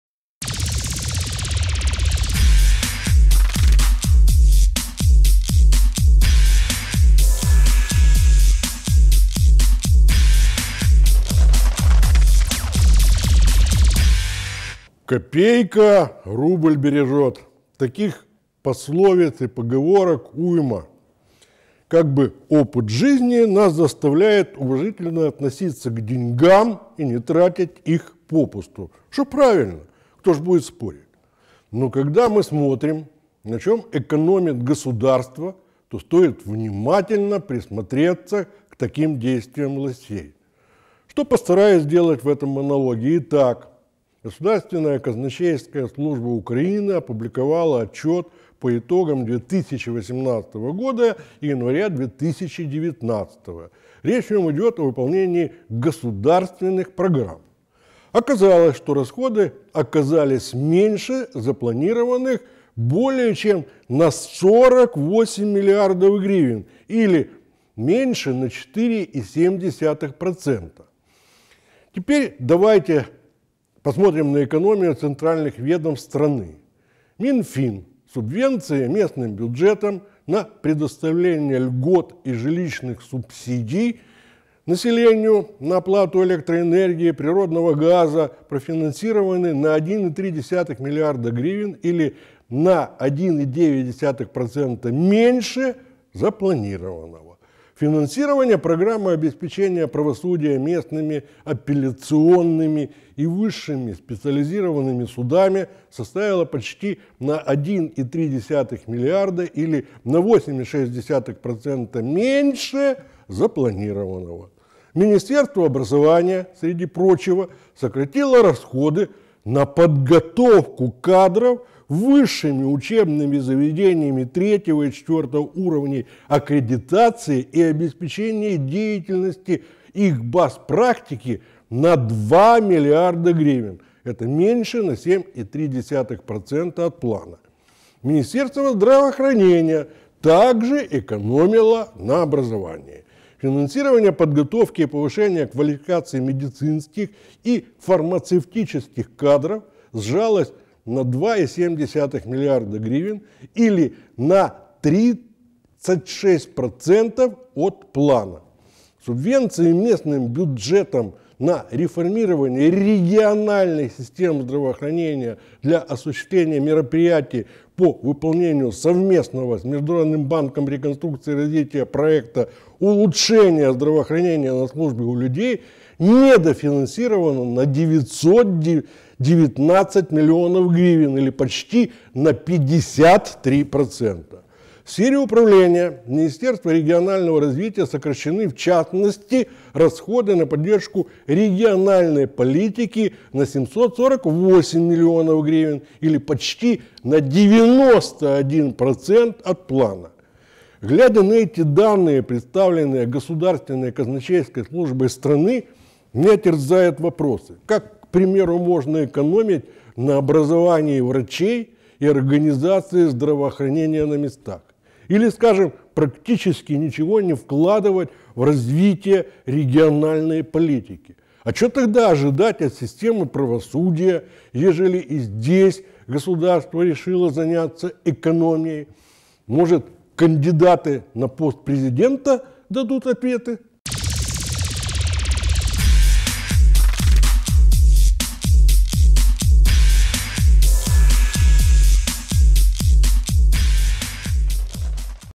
Госказначейство Украины: на чем экономим?
«Копейка рубли бережет!» - таких пословиц и поговорок уйма. Как бы опыт жизни нас заставляет уважительно относиться к деньгам и не тратить их попусту. Что правильно! Кто же будет спорить?
Но когда мы смотрим, на чем экономит государство, то стоит внимательно присмотреться к таким действиям власти. Что постараюсь сделать в этом монологе.
Итак, Госказначейство Украины опубликовало отчет по итогам 2018 года и января 2019-ого. Речь в нем идет о выполнении государственных программ. Оказалось, что расходы оказались меньше запланированных более чем на 48 миллиардов гривен, или на 4,7 процента, меньше, чем было запланировано.
Теперь давайте на экономию центральных ведомств страны. Минфин: субвенции местным бюджетам на предоставление льгот и жилищных субсидий населению на оплату электроэнергии, природного газа профинансированы на 1,3 миллиарда гривен, или на 1,9 процента меньше запланированного.
Финансирование программы обеспечения правосудия местными, апелляционными и высшими специализированными судами составило почти на 1,3 миллиарда, или на 8,6 процента меньше запланированного.
Министерство образования, среди прочего, сократило расходы на подготовку кадров высшими учебными заведениями III и IV уровней аккредитации и обеспечение деятельности их баз практики на 2 миллиарда гривен (меньше на 7,3 процента от плана).
Министерство здравоохранения также экономило на образовании: финансирование подготовки и повышения квалификации, медицинских и фармацевтических кадров, сжалось на 2,7 миллиарда гривен, или на 36 процента от плана.
Субвенция местным бюджетам на реформирование региональных систем здравоохранения для осуществления мероприятий по выполнению совместного с Международным банком реконструкции и развития проекта «Улучшение здравоохранения на службе у людей» недофинансирована на 919 миллионов гривен, или почти на 53 процента.
В сфере управления Министерства регионального развития сокращены, в частности, расходы на поддержку региональной политики на 748 миллионов гривен, или почти на 91 процент от плана.
Глядя на эти данные, представленные Государственной казначейской службой страны, меня терзают вопросы. Как, к примеру, можно экономить на образовании врачей и организации здравоохранения на местах? Или, скажем, практически ничего не вкладывать в развитие региональной политики? А чего тогда ожидать от системы правосудия, ежели и здесь государство решило заняться экономией?
Может, кандидаты на пост Президента дадут ответы?